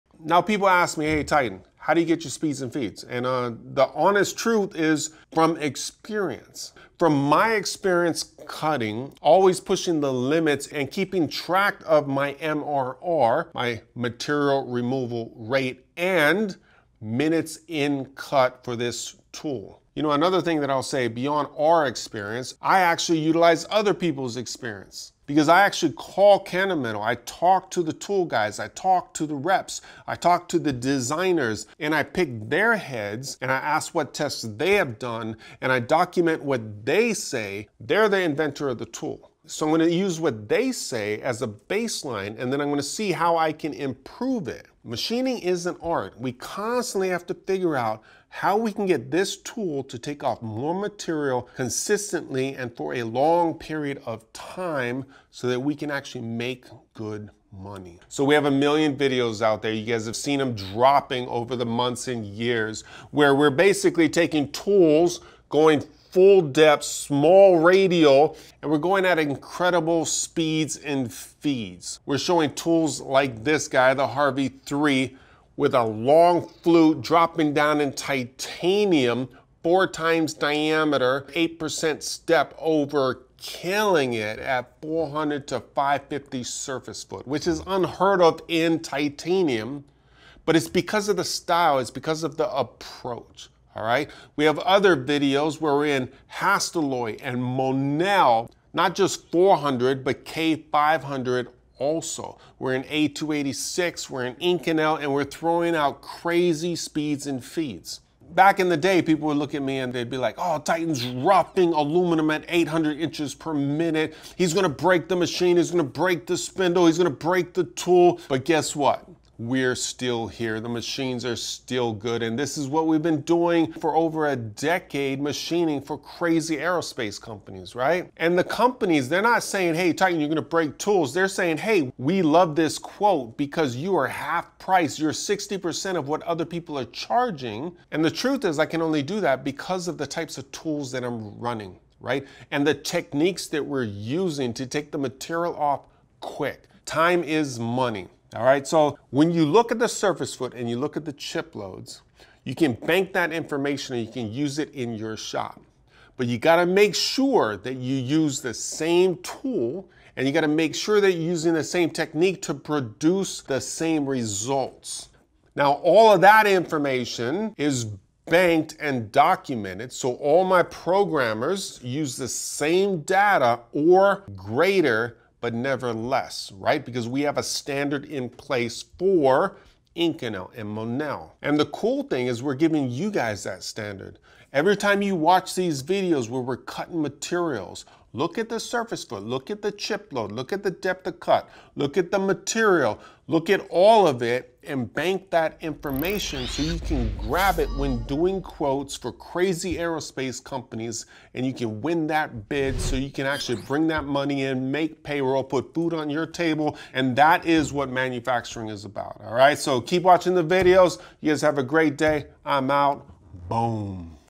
How to Get PERFECT Speeds and Feeds for Any Material in CNC Machining?
Speeds and Feeds are a HUGE part of CNC Machining… every second is money. The wrong number can break a tool or scrap your part or crash your machine! The right speeds and feeds could get you that promotion, could get you that customer, could get you everything you could possibly want in this industry!

Help support FREE industrial education by purchasing the products shown in our videos Easy purchasing, at a fair price.